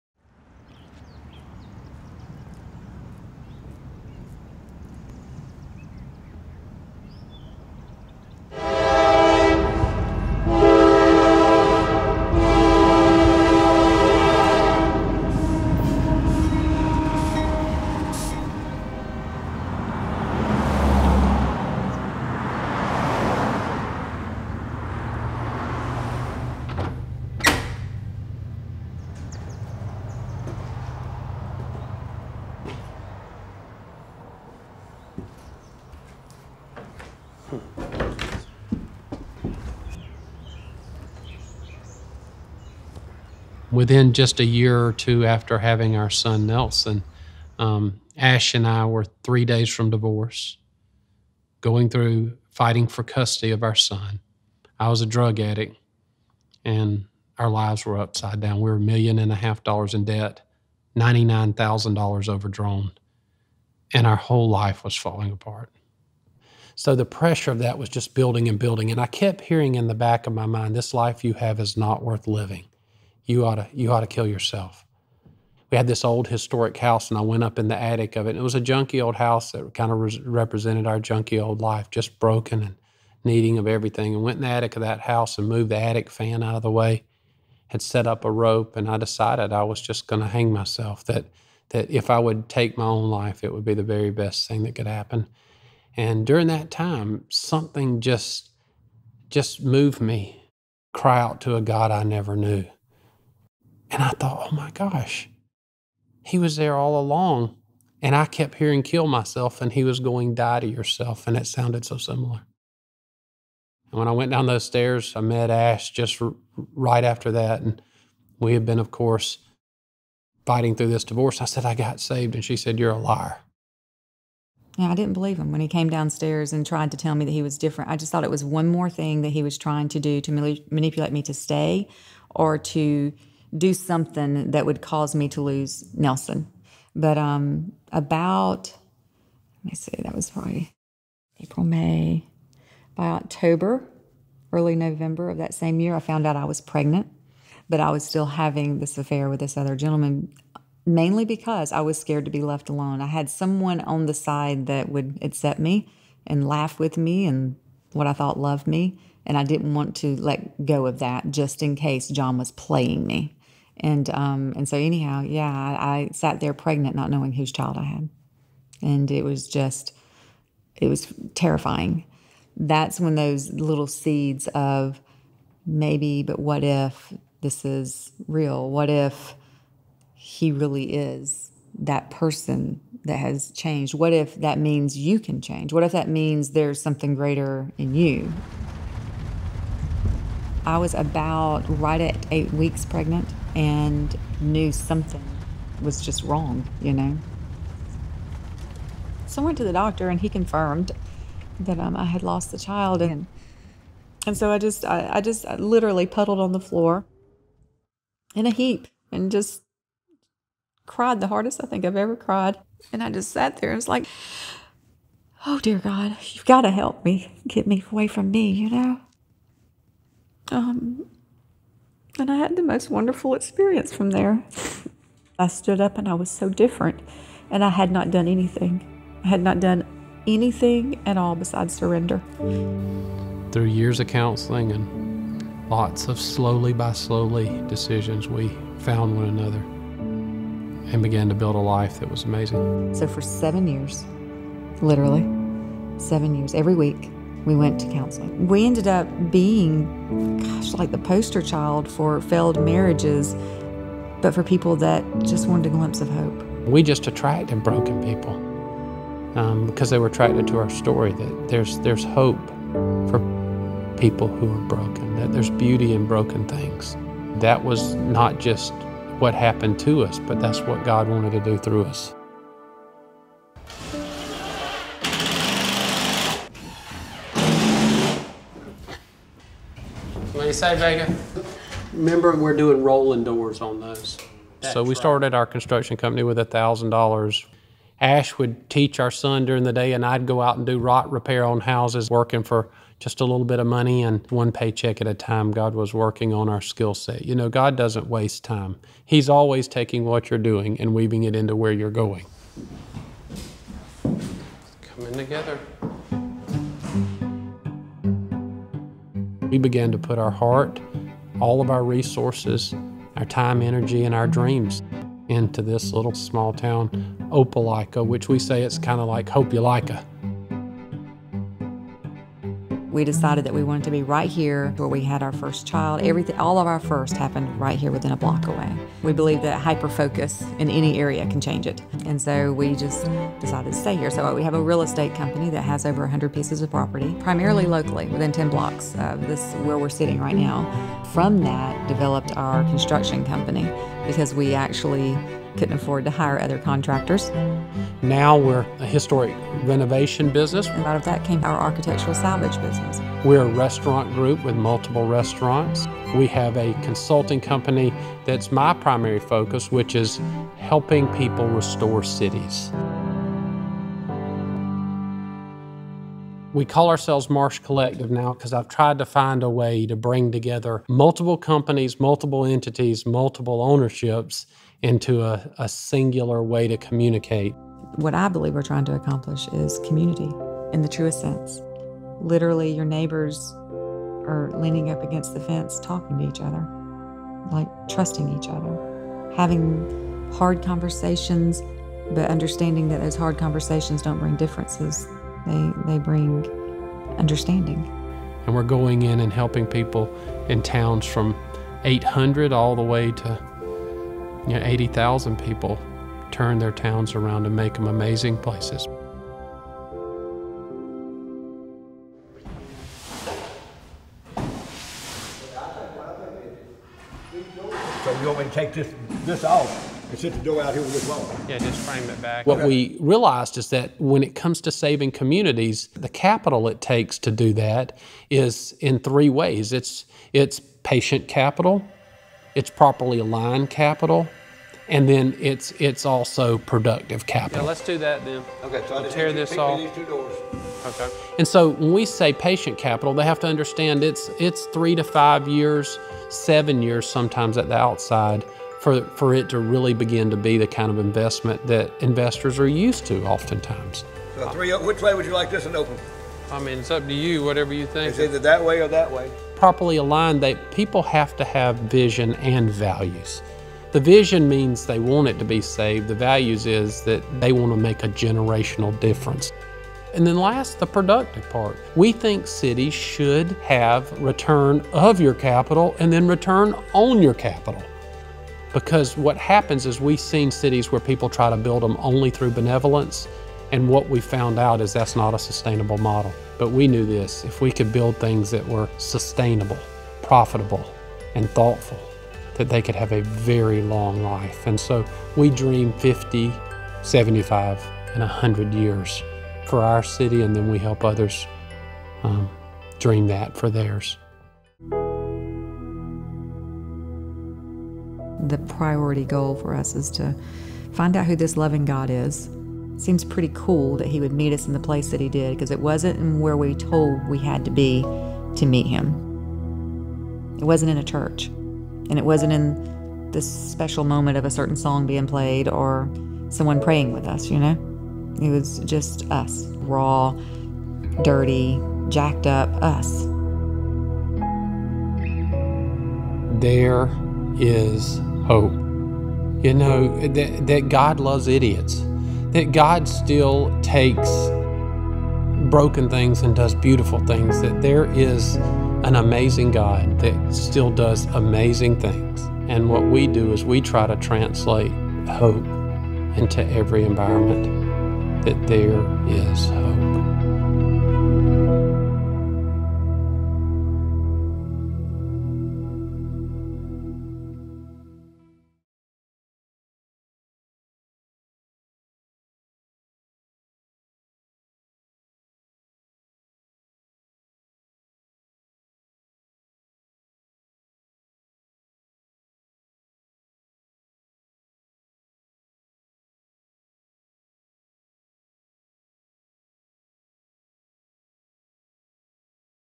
BEAUTY OUT OF BROKENESS: Marsh Collective builds hope (Faith & Co)
They are the proprietors of Marsh Collective, a conglomerate of business ventures dedicated to resurrecting the city of Opelika, Alabama. Through a vulnerable recounting of their past tumult, the Marshes share how healing in their broken lives became their inspiration to bring hope to others through business.